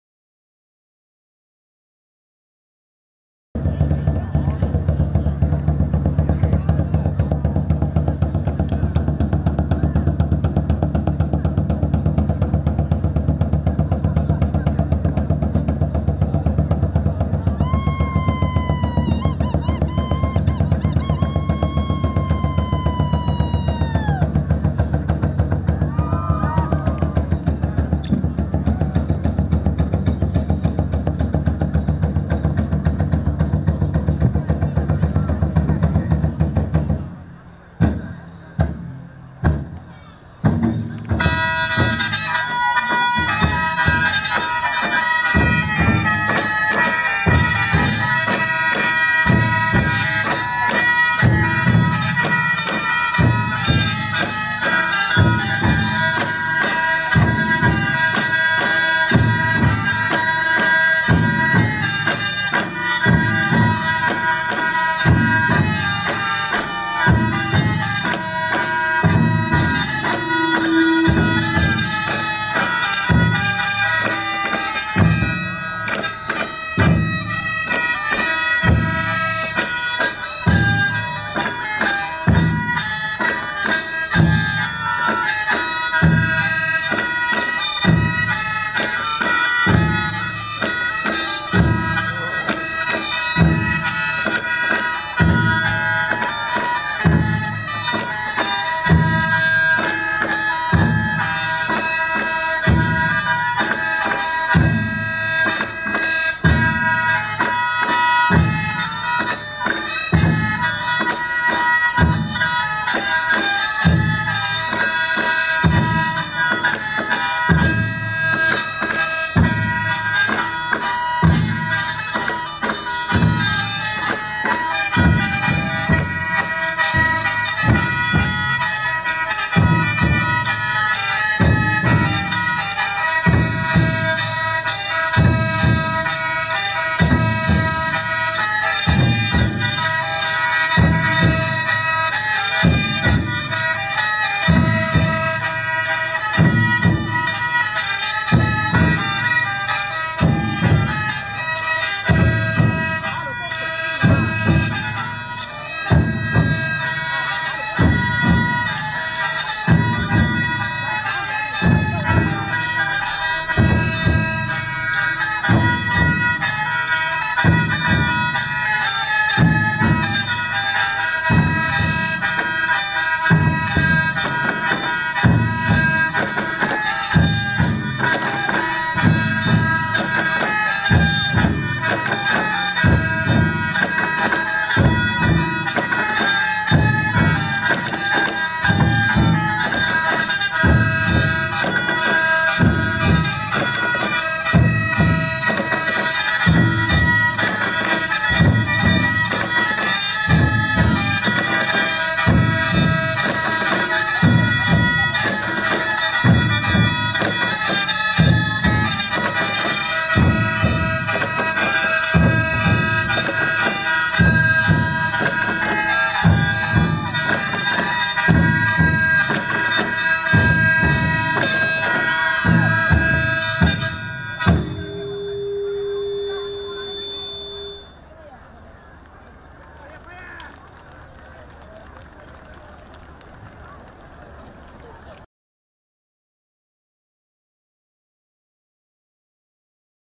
Hmoob Hauvtoj Ntiajteb, Suav Teb, Dej Liab: Peb Ntxhais Hmoob Seev Cev 苗族国际花山节
2014 中国红河苗族国际花山节。